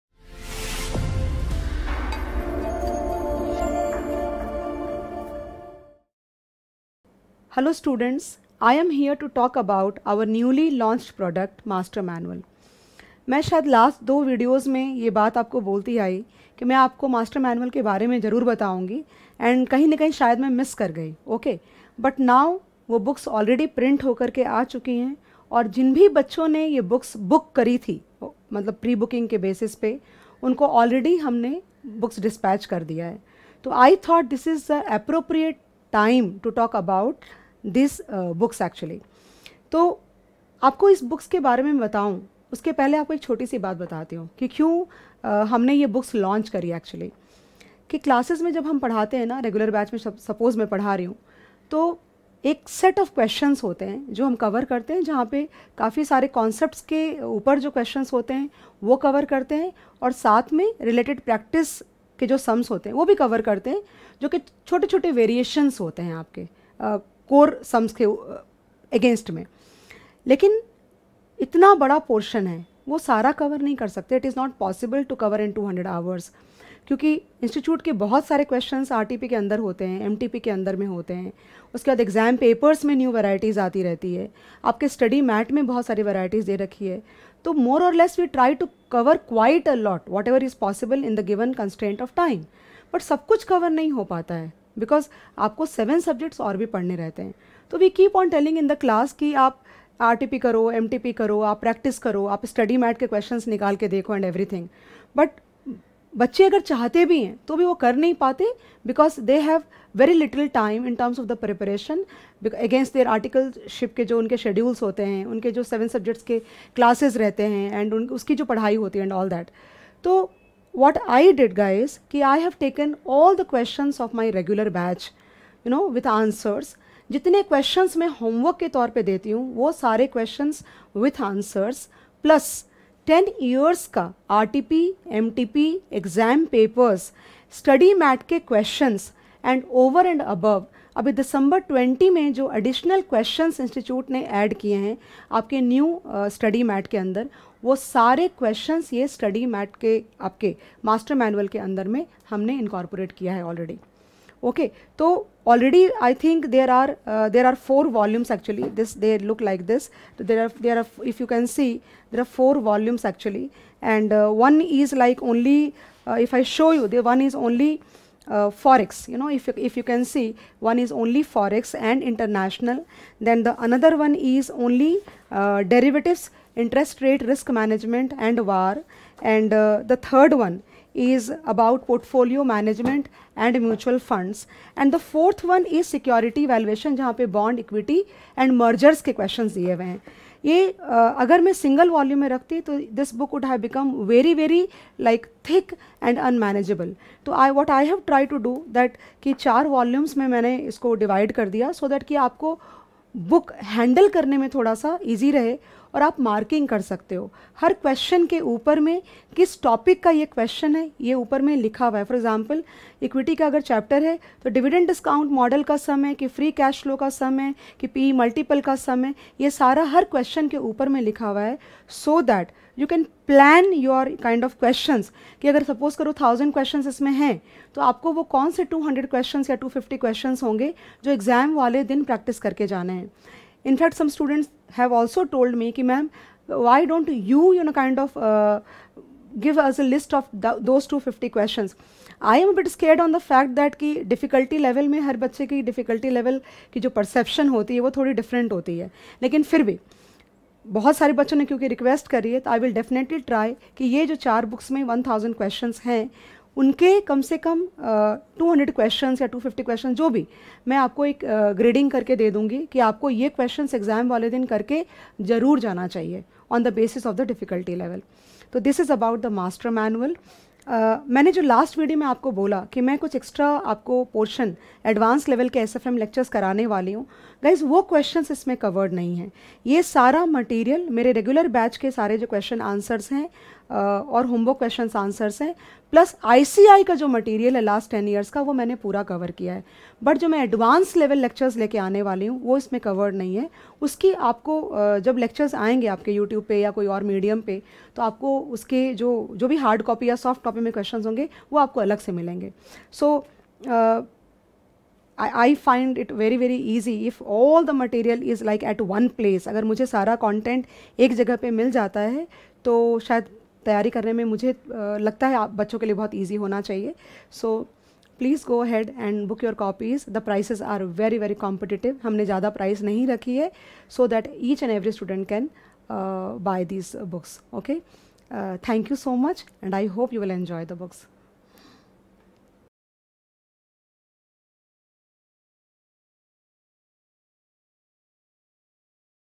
Master Manual by Prof. Archana Khetan | CA Final SFM (ICAI)
Link to the SFM Master Manual Product on Website
- SFM Master Manual - Rs 1200 only

SFM Master Manual is an exhaustive compilation of questions & answers from Strategic Financial Management  (SFM CA Final) course. It Covers approximately 1000  Questions with solutions whereas CA Final SFM Regular Course covers approximately 500-600 questions.
SFM Master Manual is  divided into 4 Volumes.
1. Forex & International  Financial Management
2. Derivatives, Interest Rate Risk Management & Risk Management (VAR)
3. Portfolio Management & Mutual Funds
4. Security Valuation  ( Bonds & Equity) & Merger  & Acquisitions

Features of SFM Master Manual
a. Exhaustive Coverage - This book  covers all possible varieties both in terms of teaching content of                   Prof. Archana Khetan as well as ICAI content.

b. Cross-Checks  - All sums are with solutions, and hence students can have a quick cross check.

c. Latest Content – Covers latest sums added by ICAI in Dec 2020 as Amendments, the same have been added and duly tagged.